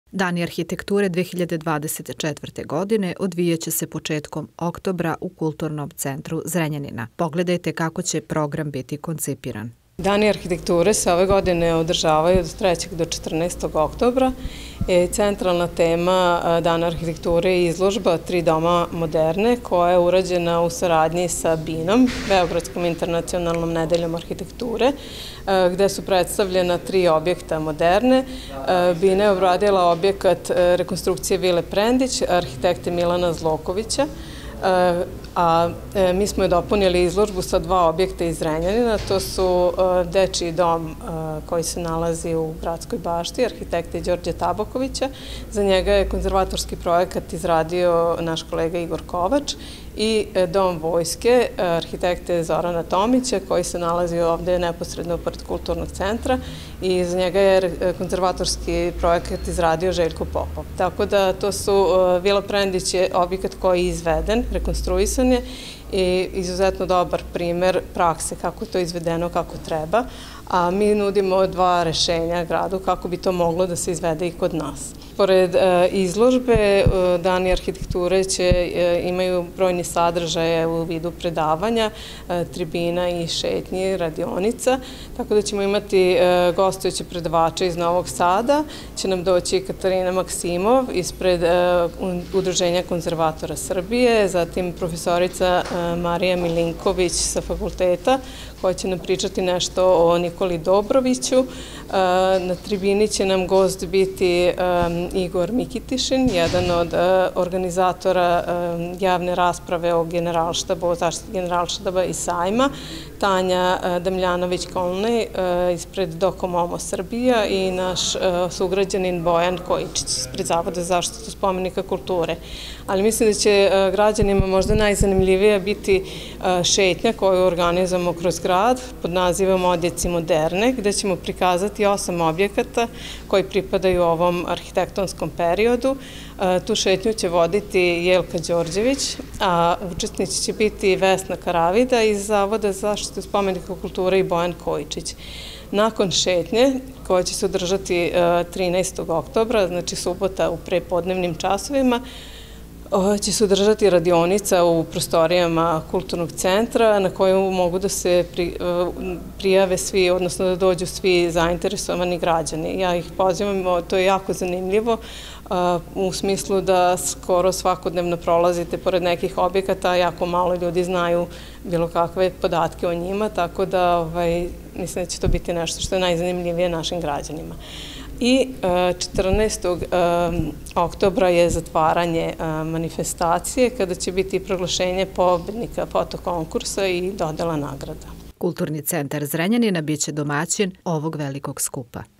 USKORO DANI ARHITEKTURE 2024.
Dani arhitekture 2024. odvijaće se početkom oktobra u Kulturnom centru Zrenjanina. Raznovrsan sadržaj nas očekuje tokom ovogodišnjeg okupljanja, a u nastavku pogledajte kako će biti koncipiran program, kao i šta će bitibpredstavljeno svim posetiocima.